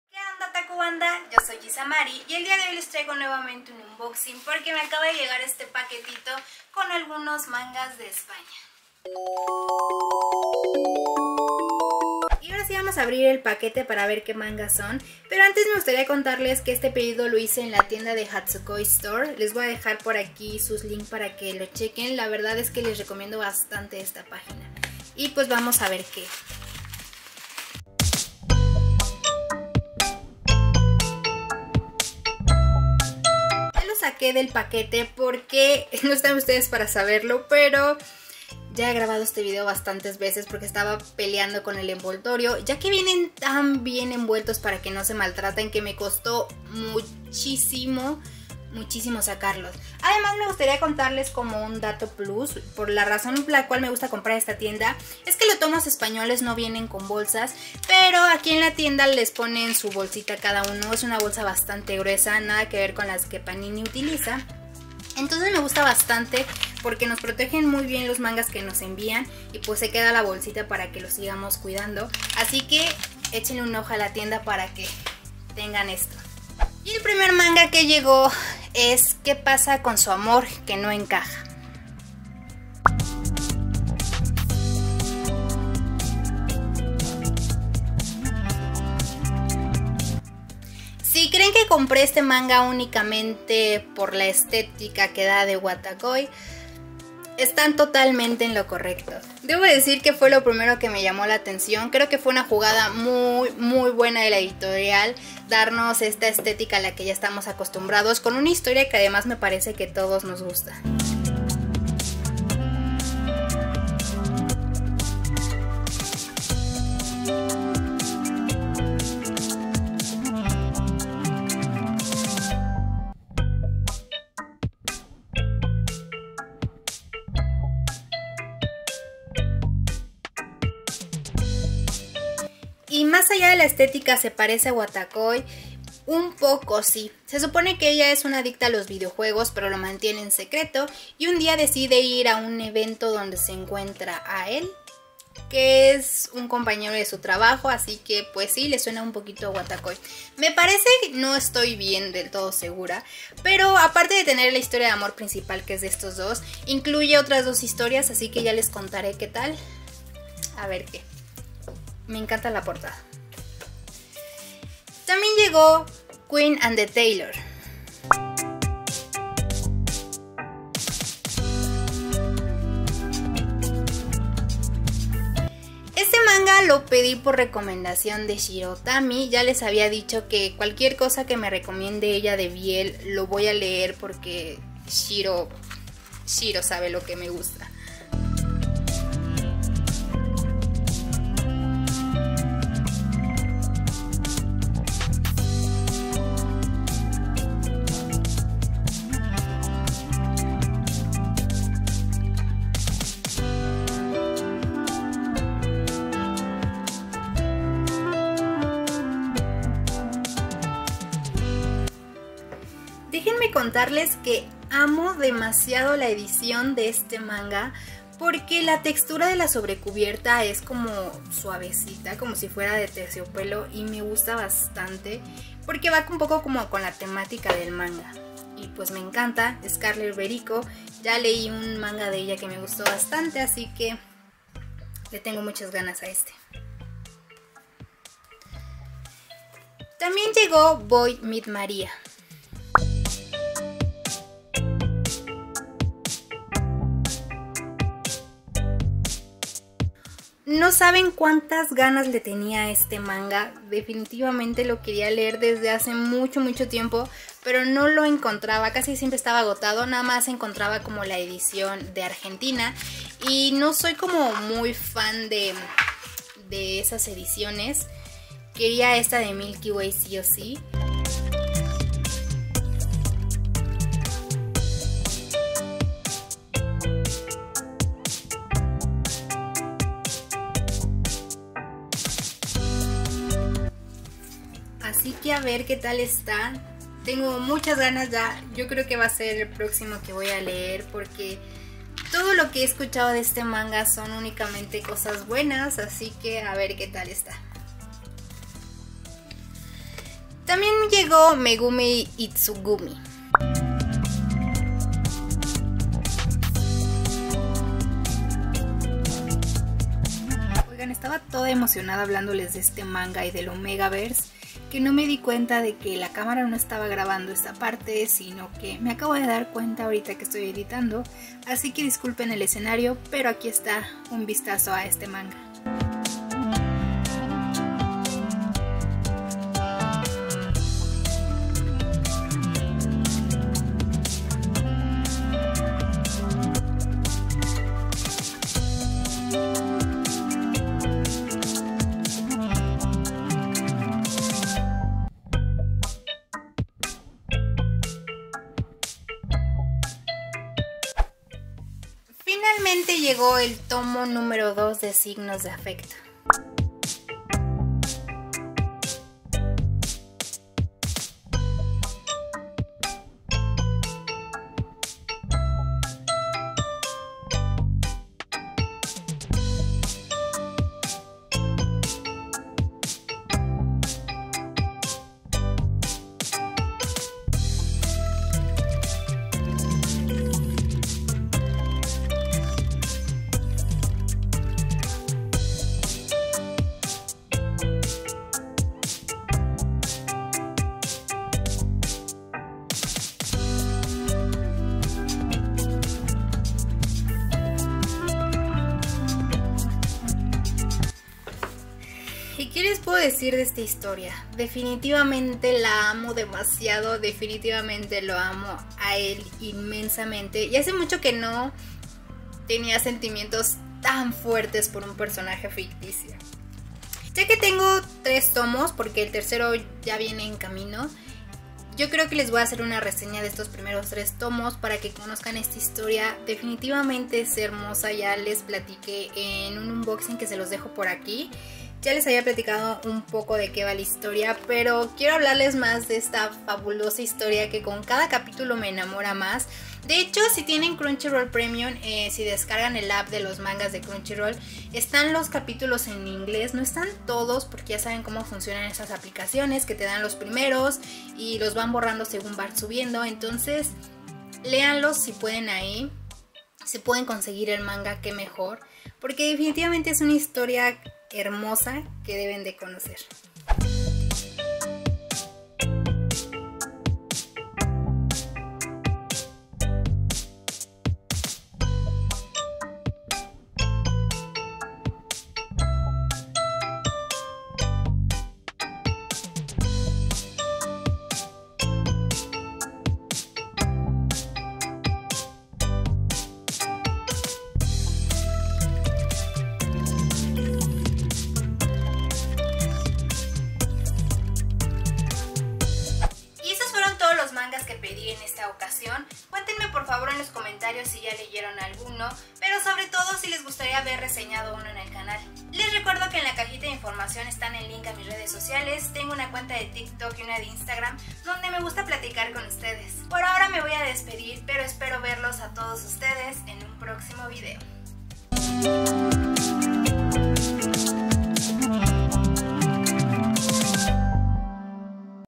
¡Llegaron mangas de España! // Omagaverse + BL + amores de oficina😘
¡Hola, otaku banda!
(灬º‿º灬)♡

El día de hoy les traigo un unboxing de unos mangas que me llegaron de España a los cuales les tenía muchísimas ganas 💕😍

💕 ¡VAMOS A SEGUIRNOS! 💕